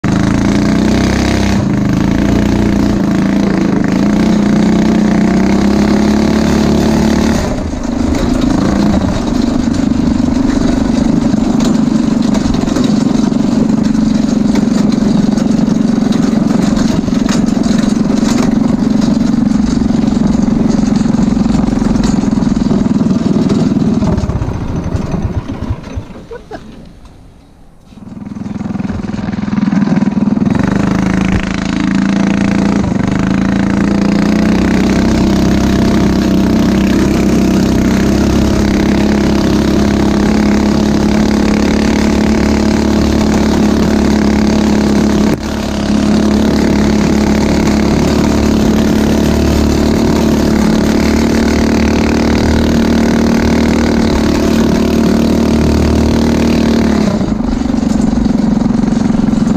Manco Rattler LXT Rebuild - First Ride
Here's a video of a Manco Rattler that I've recently Rebuilt, Viewpoint from my 16 year old brother.
Manco RattlerLXT,  an upgraded Predator 212 with 30 series torque converter, 22in Carlisle Strykers, and a 42 tooth sprocket (great for speed, but going with a 64 soon for trail riding)
Kart made possible by GoPowerSports: